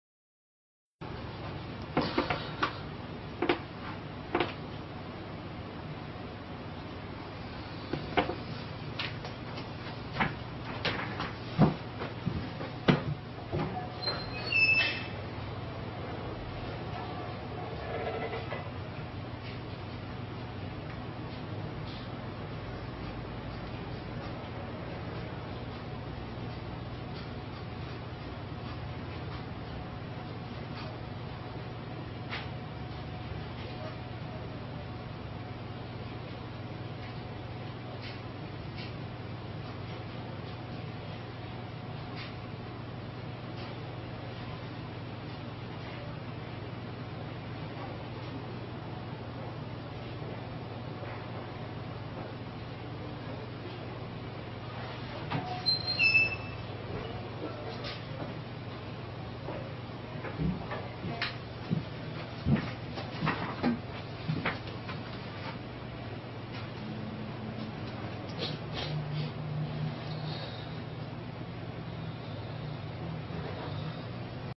Roban un cajero en menos de un minuto
Las cámaras de vigilancia captaron el momento en el que un grupo de ladrones irrumpió en un banco y logró robar un cajero en tan solo 59 segundos.